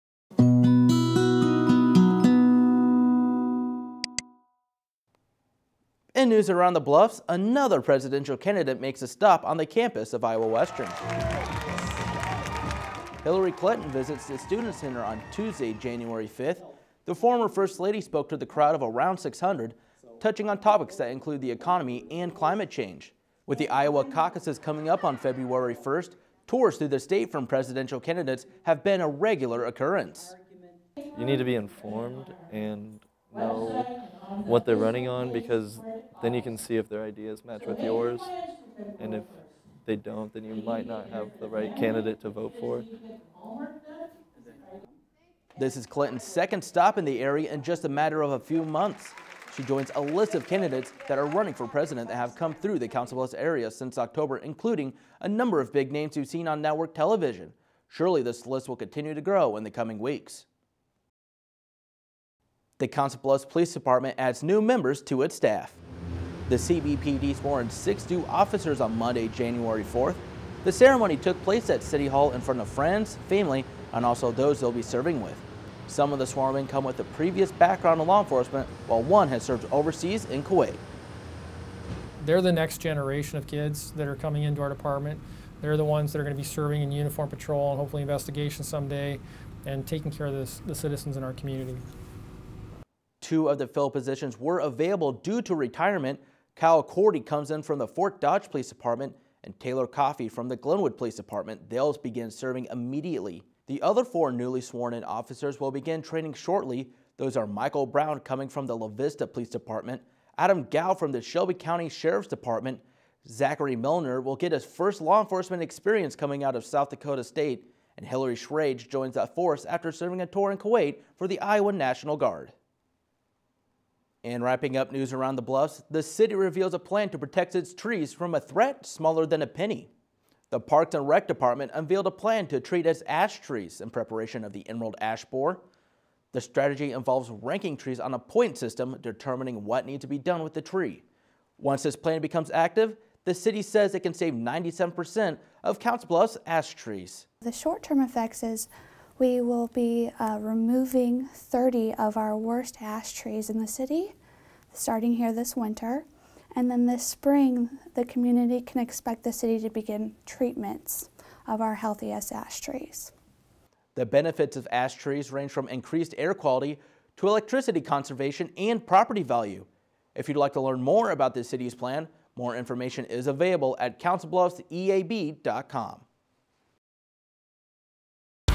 News Around the Bluffs 1-12-16
Hillary Clinton returns to Council Bluffs, the CBPD adds six new faces to its staff, and CB Parks & Rec unveils a plan to combat the Emerald Ash Borer